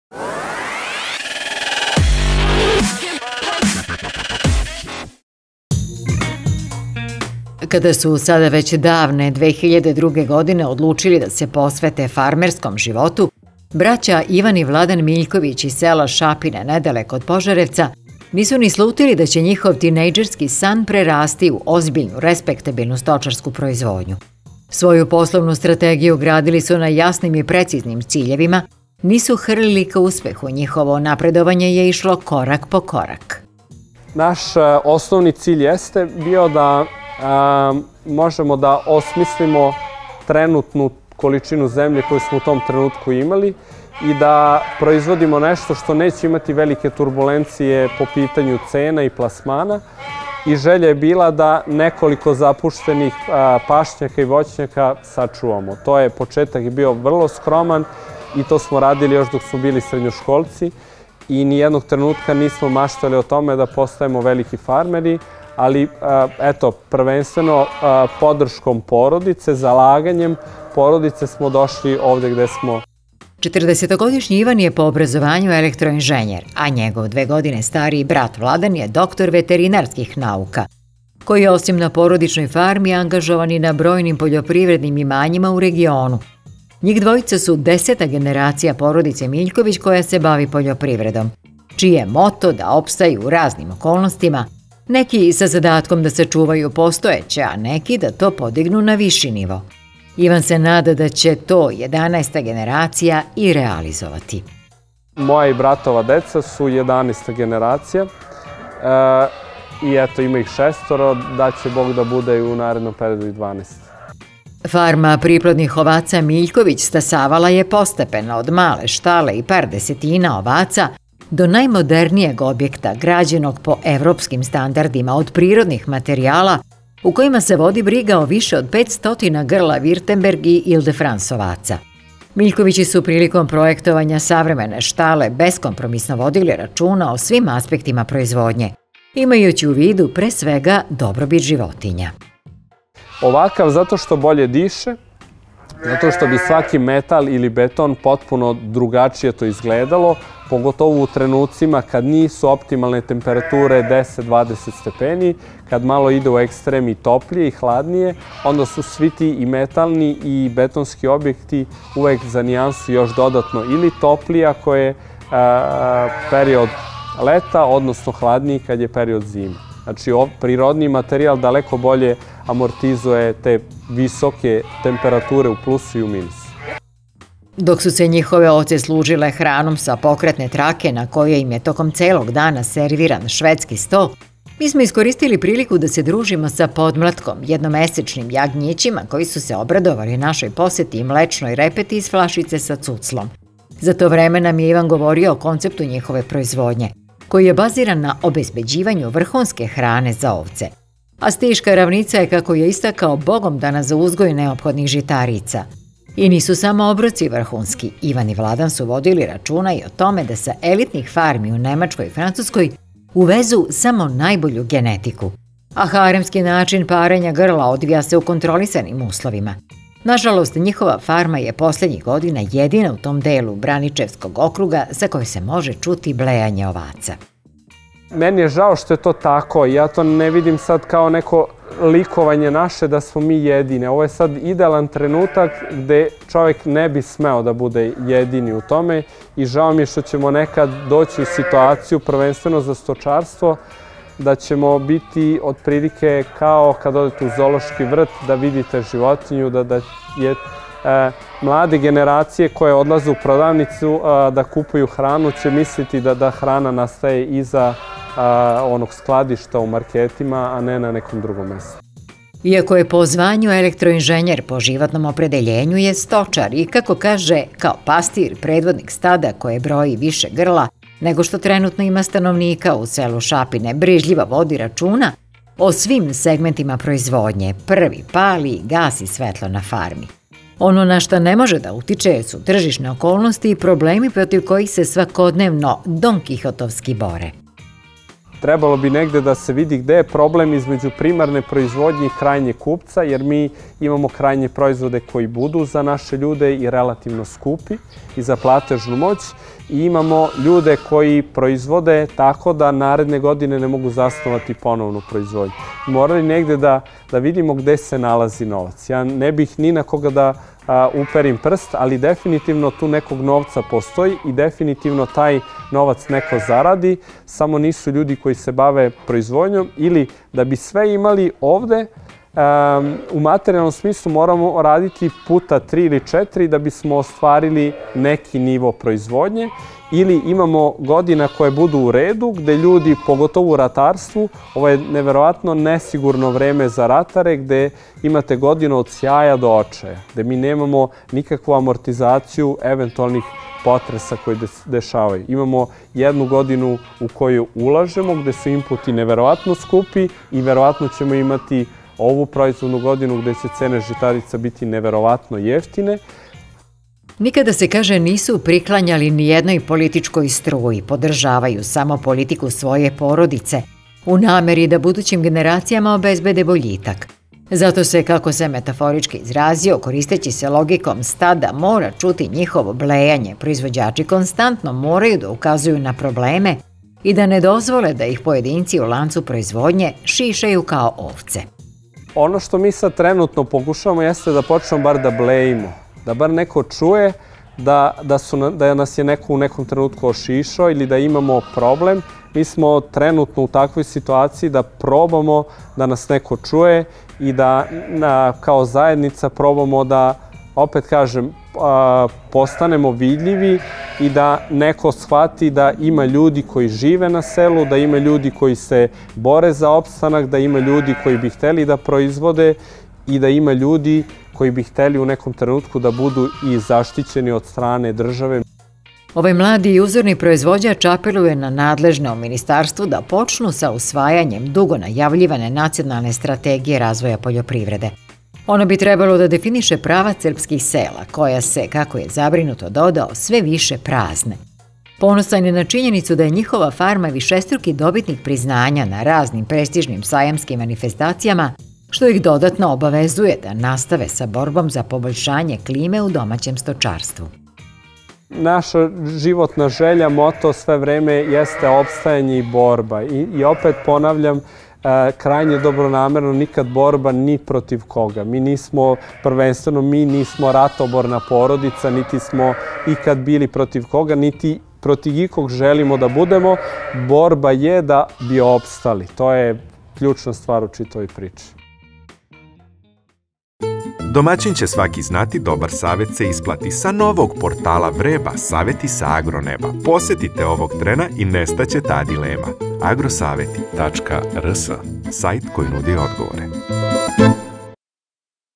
Uzgoj VIRTEMBERG i ILE DE FRANCE rase ovaca u selu SAPINE - U nasem ataru 1007
Pogledajte i ostale priče iz kategorije Ovcarstvo